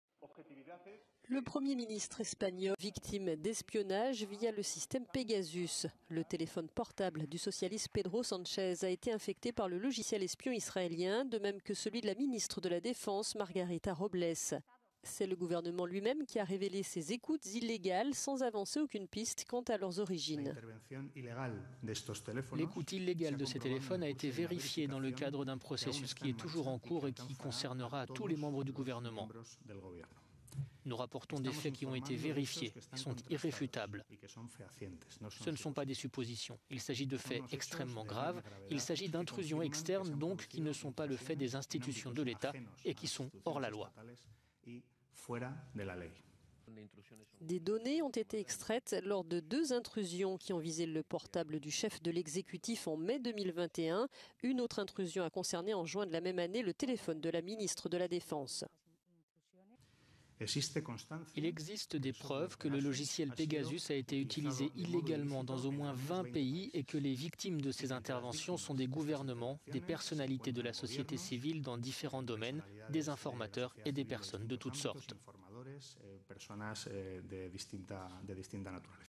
🟥Espagne :les téléphones du 1er ministre et la ministre de la Défense victimes d'écoutes par ...?😉
ça explique peut-être le changement de position de l'Espagne sur le Sahara Occidental 🇪🇭